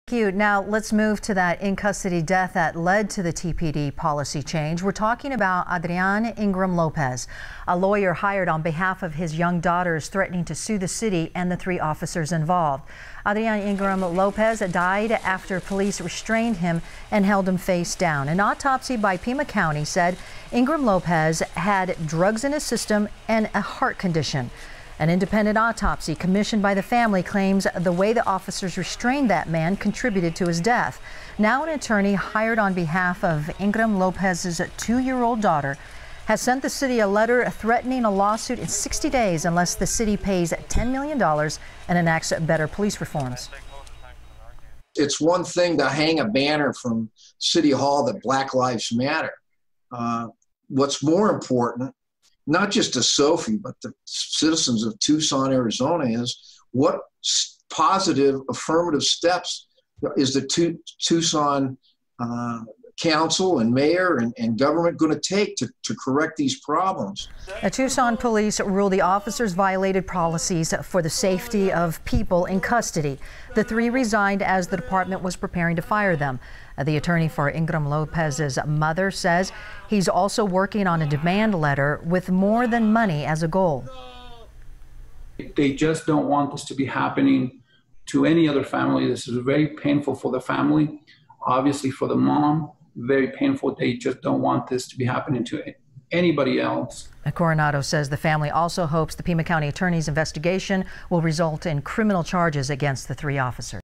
$10 Million lawsuit threatened in TPD in-custody death
Adrian Ingram-Lopez died while restrained by police